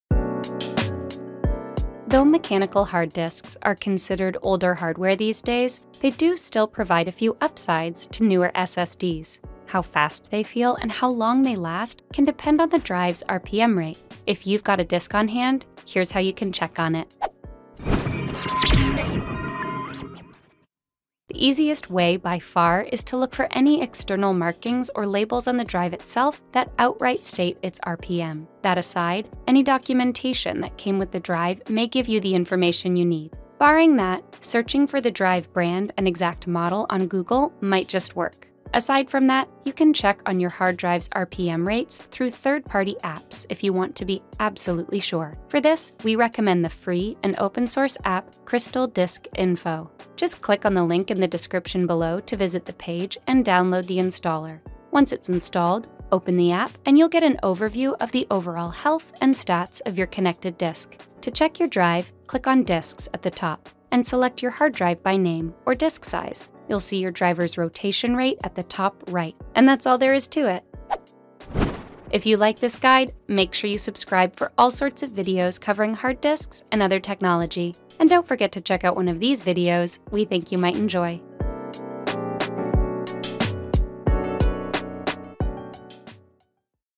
How to Check Hard Drive RPM Rates
Though mechanical hard disks are considered older hardware these days, they do still provide a few upsides to newer ssds. How fast they feel, and how long they last can depend on the drive’s RPM rates. If you’ve got a disk on hand, here’s how you can check it!